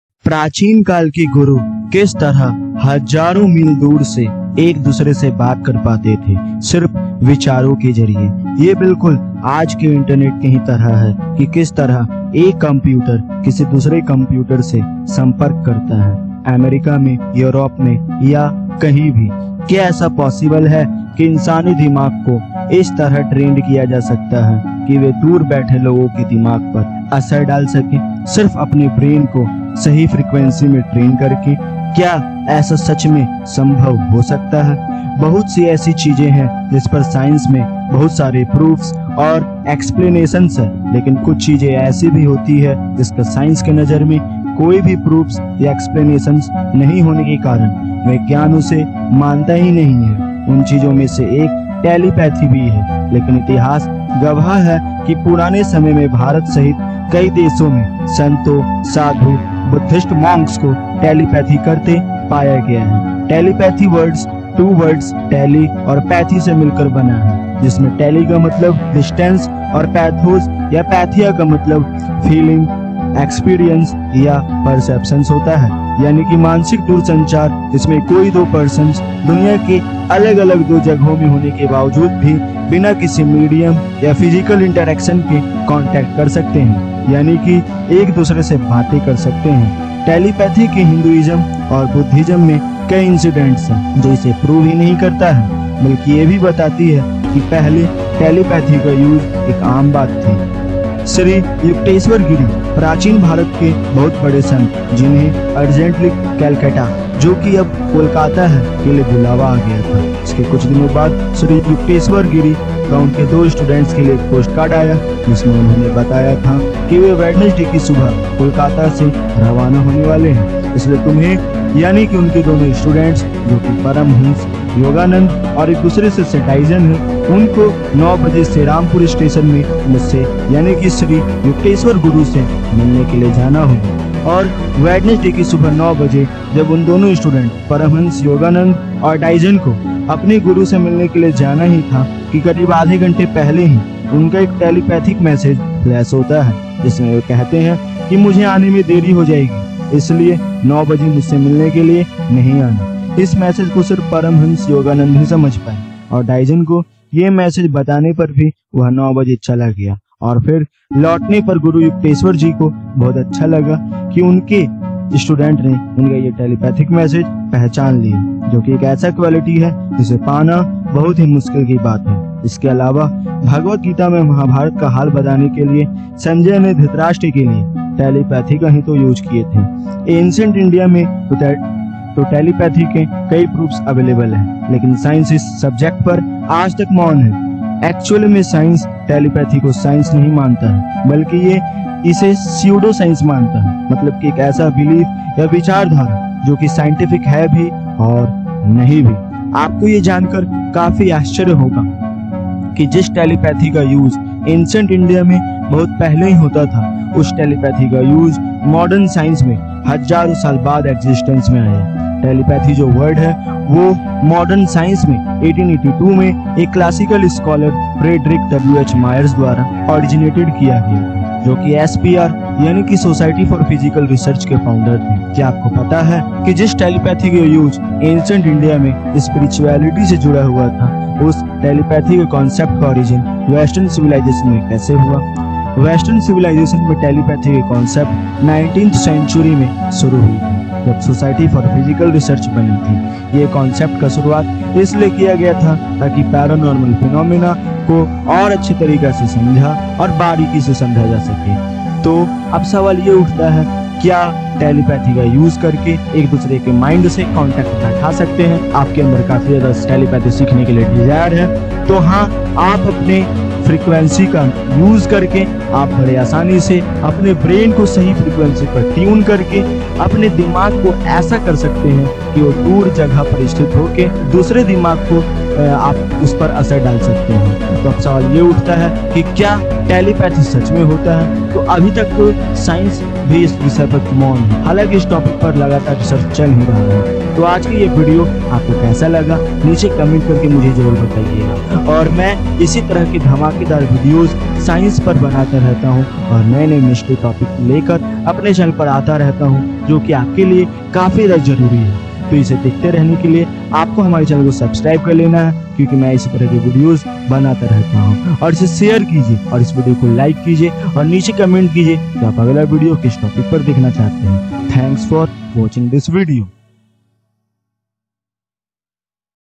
What is Telepathy and How to Learn? | Alltechfacts
What is Telepathy and How to Learn?

Telepathy is by which a user can read or sense another person’s thoughts and communicate with them mentally Basically the telepathy word divided in to two parts first one is ‘tele’ which mean distant and 'another is ‘pathy’ which means feelings. In simple words a person can send thoughts to the person. The goal is to relax your mind and body completely.